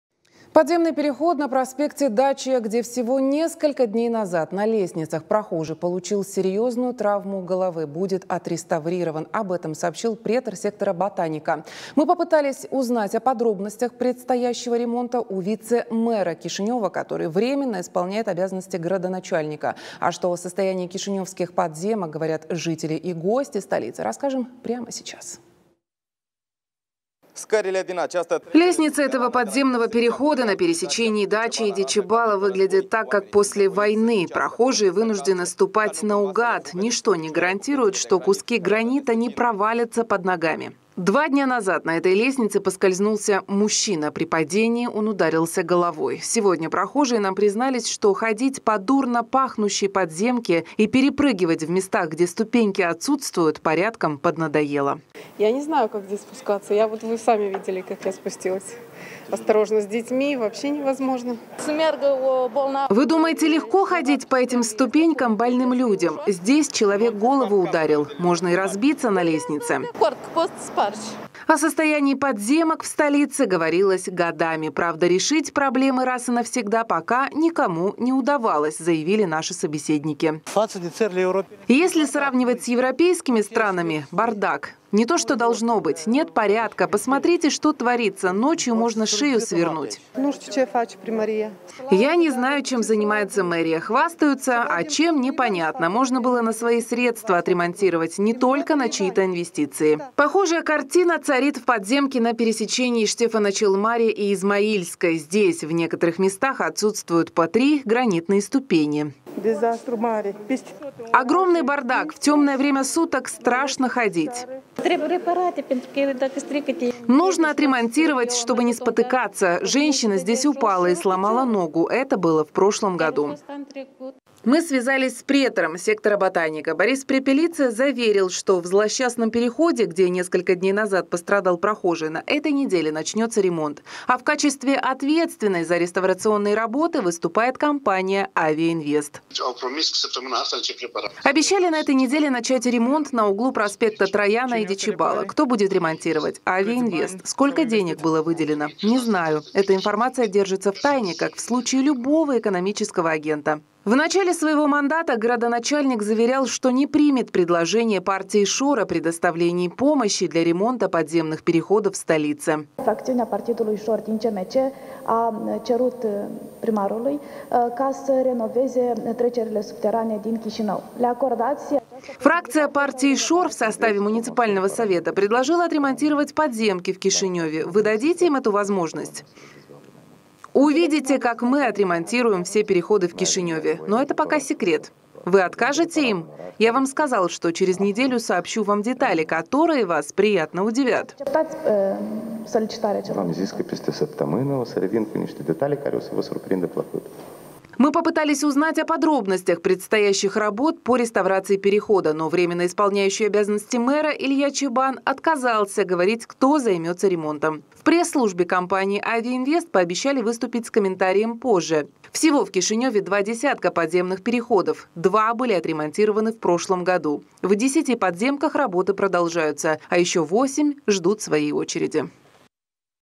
Ремонт после инцидента
Подземный переход на проспекте Дачия, где несколько дней назад на лестницах прохожий получил травму головы, будет отремонтирован. Об этом сообщил претор сектора Ботаника. Мы попытались узнать о подробностях предстоящего ремонта у вице-мэра Кишинева, который временно исполняет обязанности градоначальника. А что о состоянии кишиневских подземок говорят жители и гости столицы, расскажем прямо сейчас.

Поддержи команду TV8!